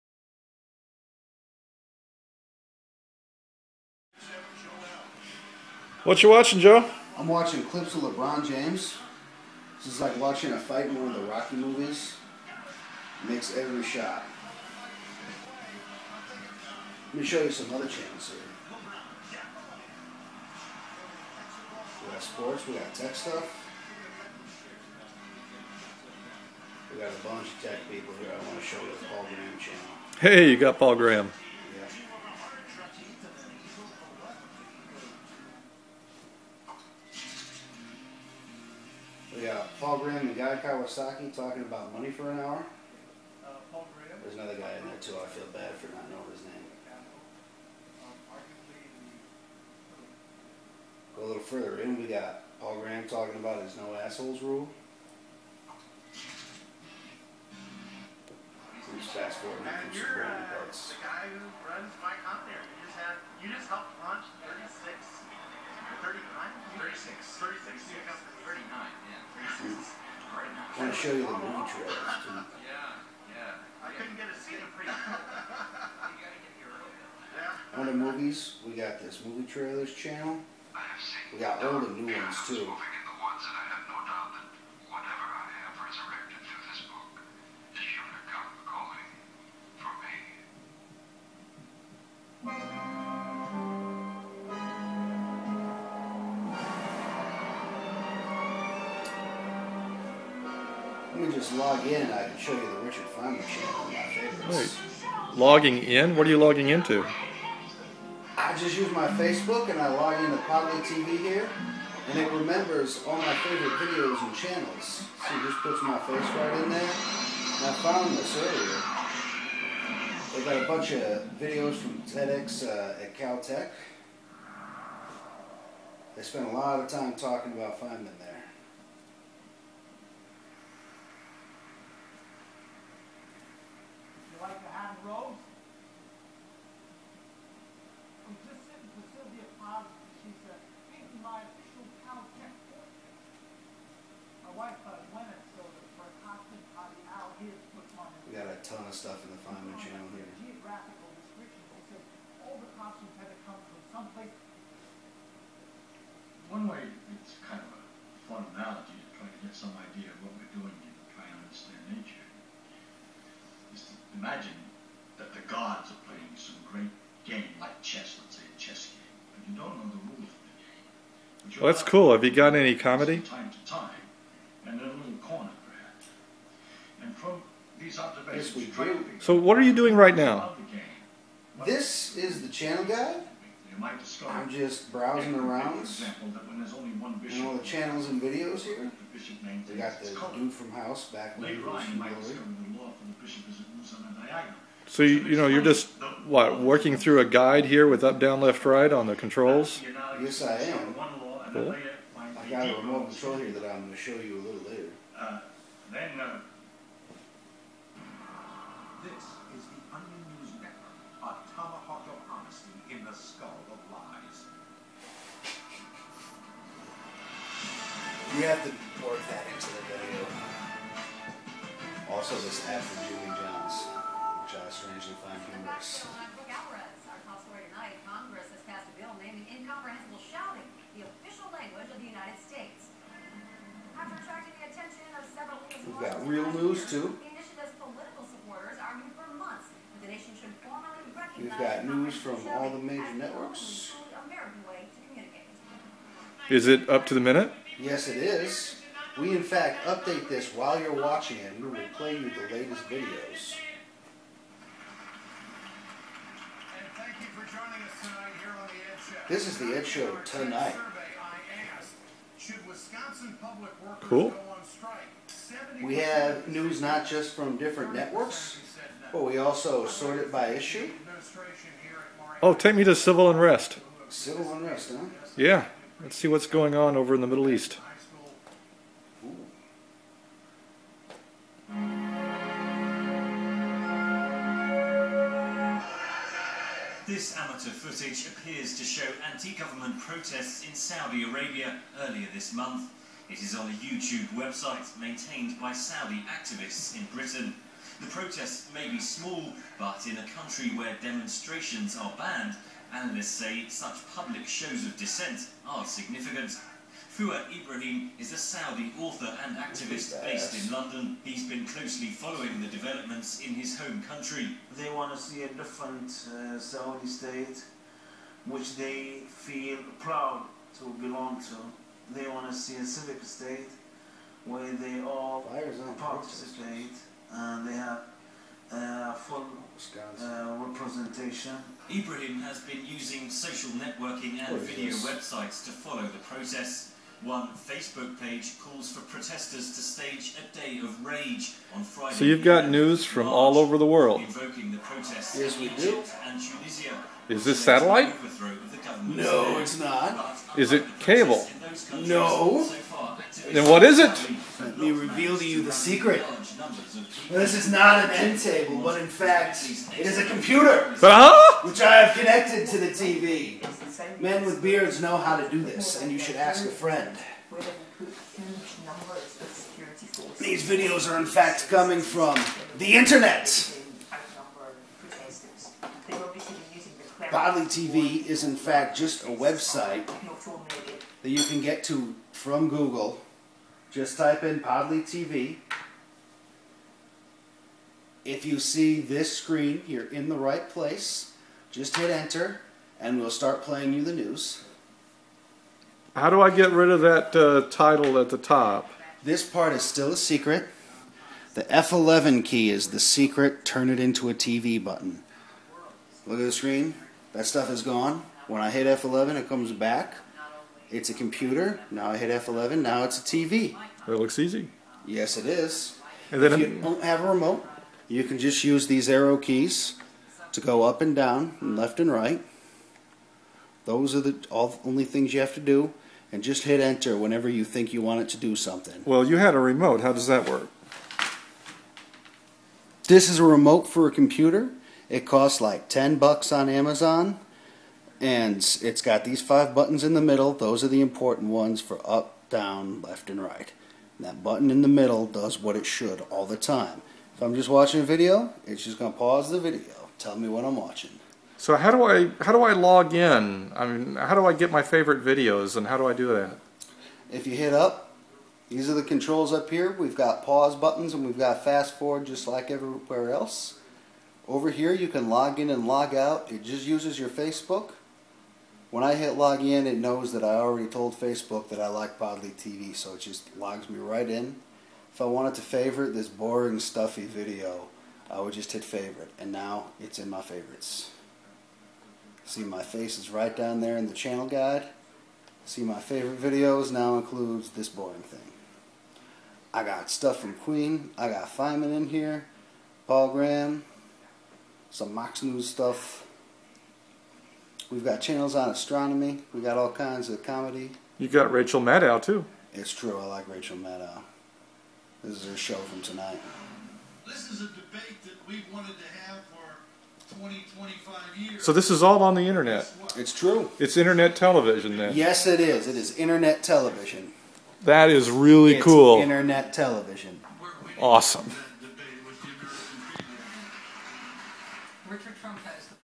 A quick tour of Podly TV (Mobile Size)
Podly TV is an Internet television service which runs in your web browser. Just connect a computer to your television, and Podly TV gives you access to thousands of thousands of interesting channels and videos from the Internet.

and give it a try. If you have suggestions or comments, send email to david, joe, or ron at metadatalabs.com.